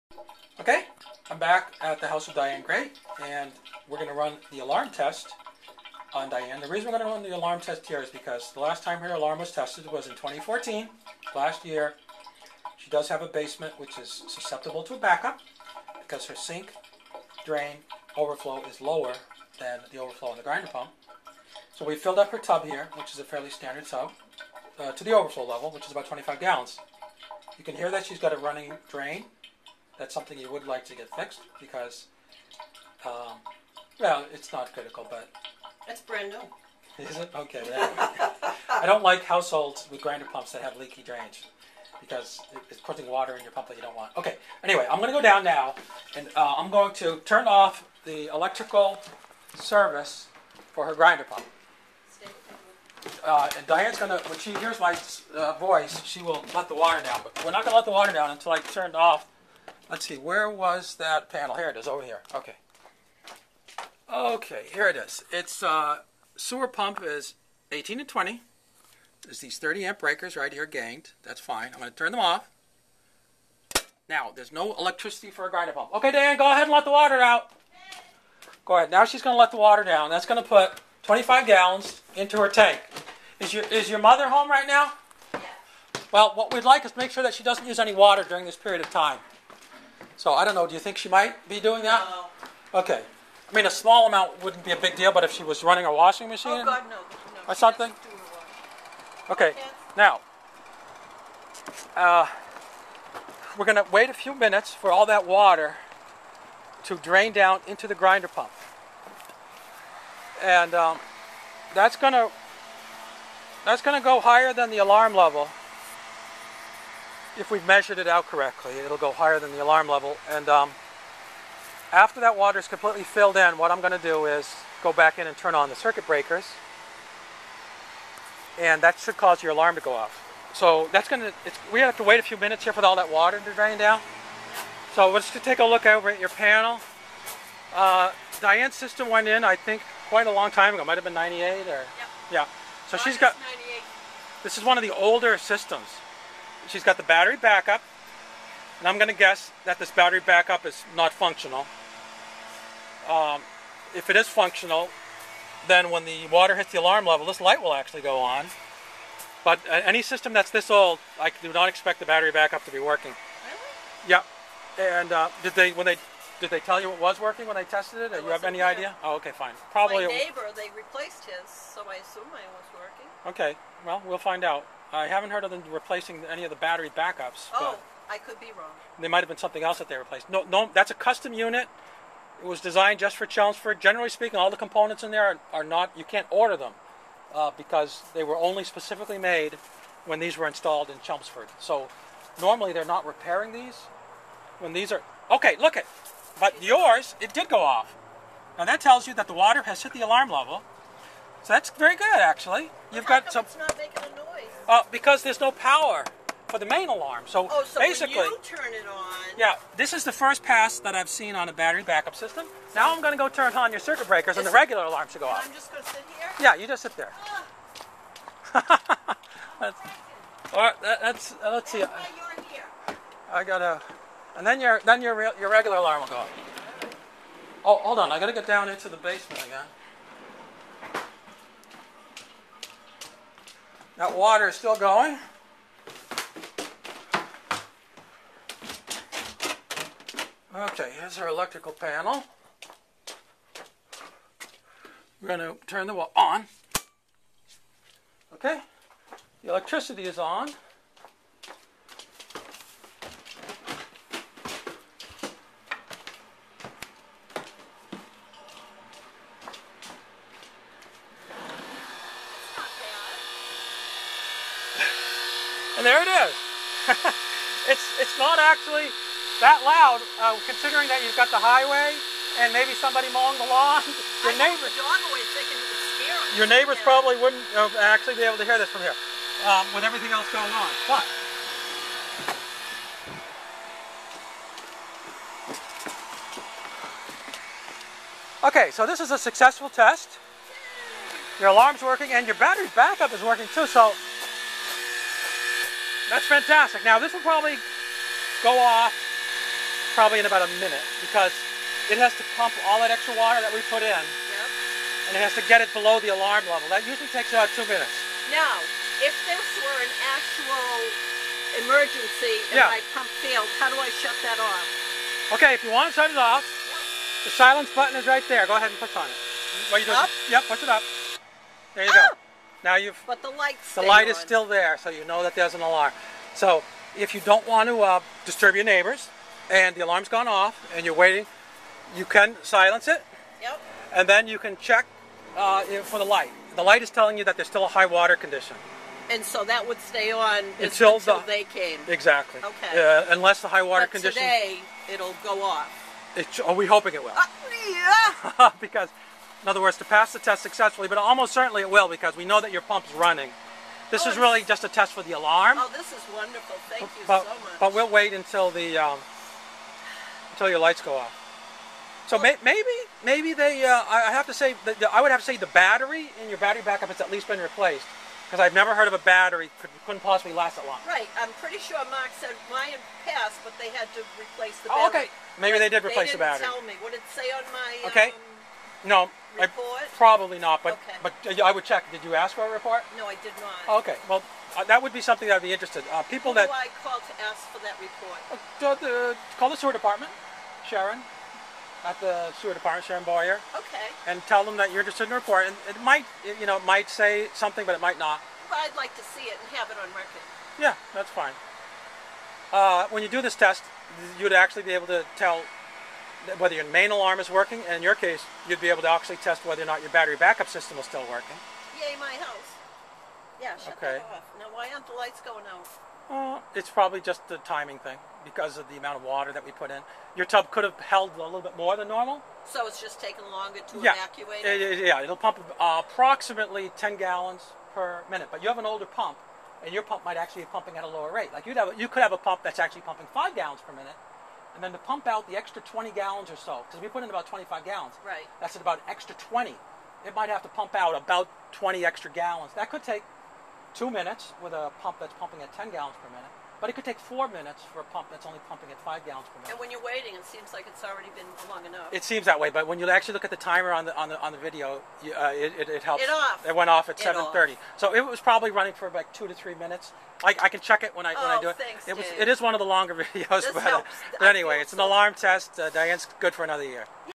Grinder Pump alarm test on oldest system to date
Diane Gray's grinder pump was installed in 1998 and is the oldest system that I have run an alarm test on. Diane has a sink in her basement which will overflow at a level which is still about 1 foot below the overflow tube on her grinder pump. This means her basement is susceptible to a backup and it is important for her to know that her alarm system is working properly.